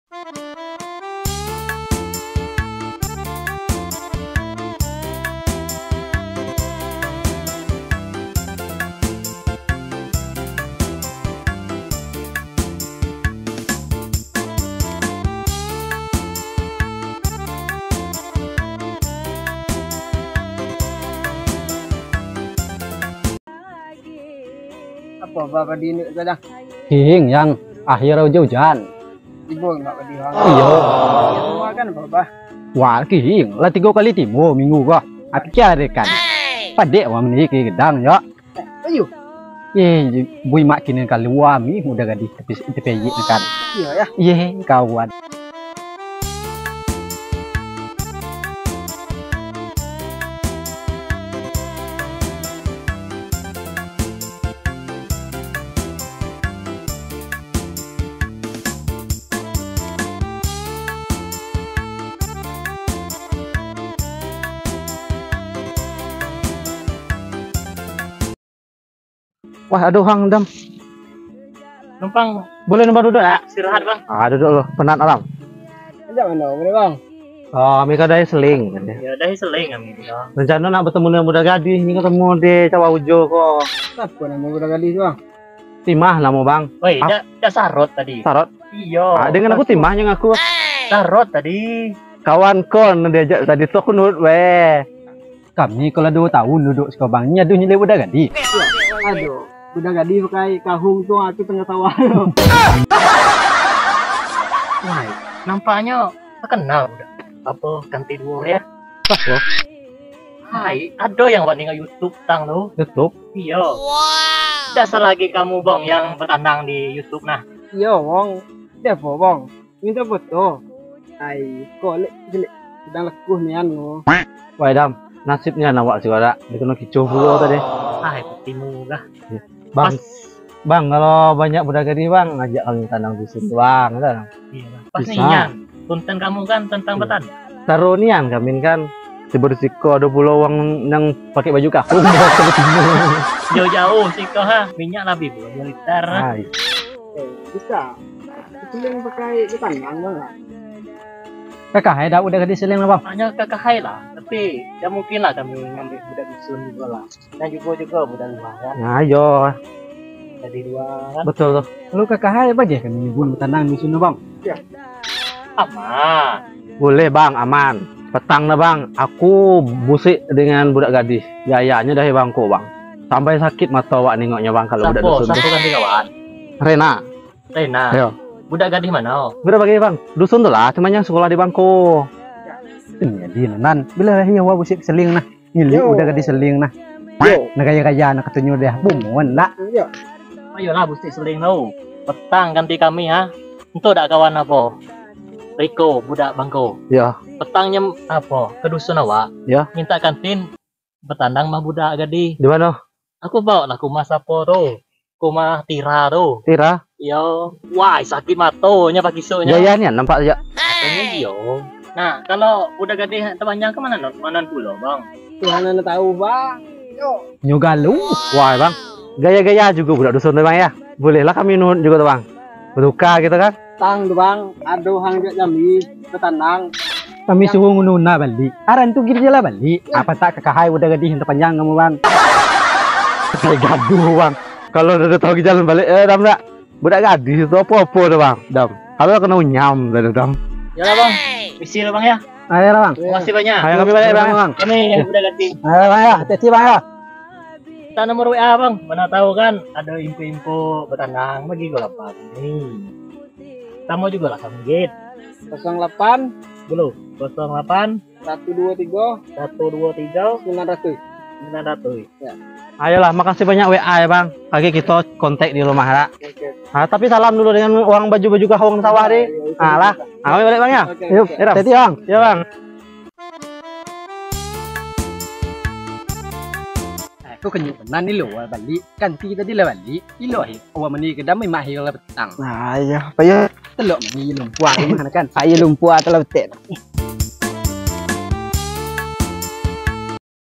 Gadis Rantau Panjang VS Gadis Seling
Cerita masalah Tradisi Betandang, Gadis Dusun dan Gadis Seling Mana yang Lebih Cantik.
Mohon maaf apabila ada kesalahan dalam penyusunan Video ini,
Semoga Terhubur
Terima Kasih